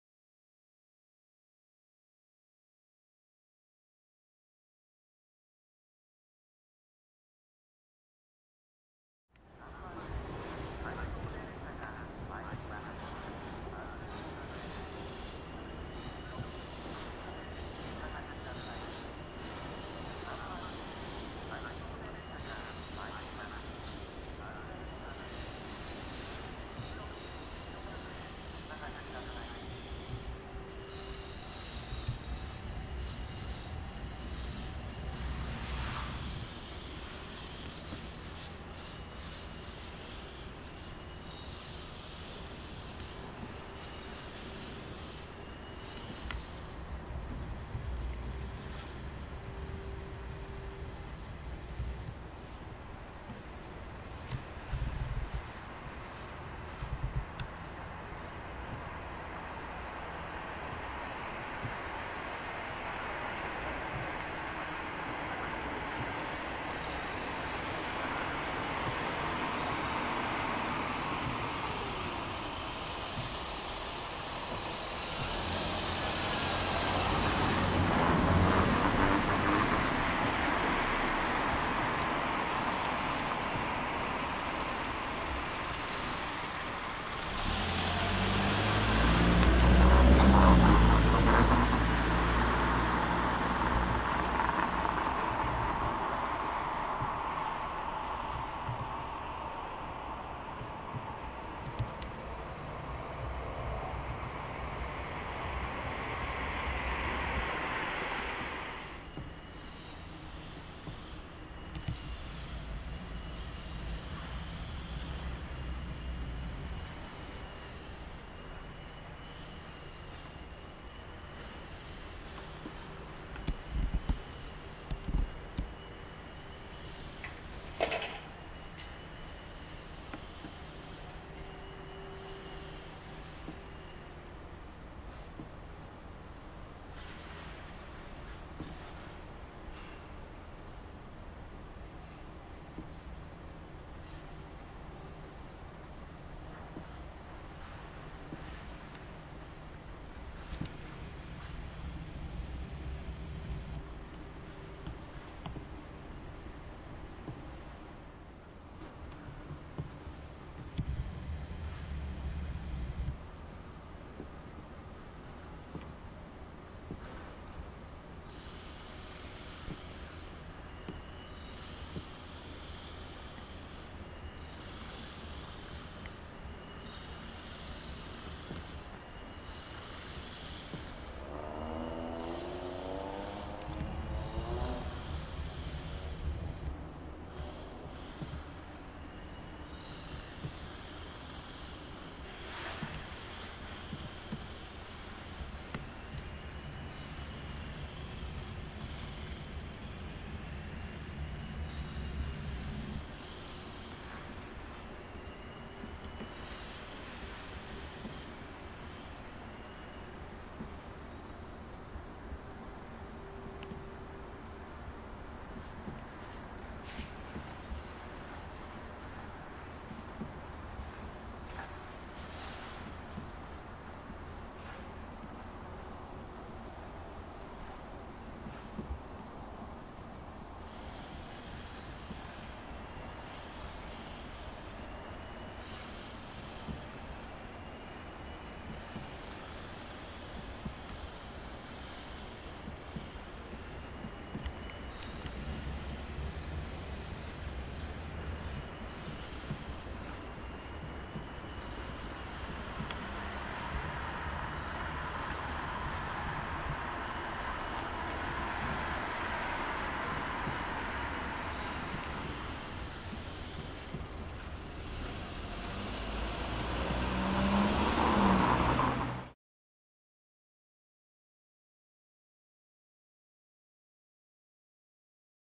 【東武 70000系】 羽生発 南栗橋行き LED表示あり これは乗りたいね!!
引上線を発車し、伊勢崎線羽生駅の下り本線に到着。
ここで折り返し、上り方面に出発します。
引上線からは無表示で到着。そのまま行くのかと思いキヤ、南栗橋に。
このまま行っちゃうのか・・・と見ていると、そうはいきませんでした。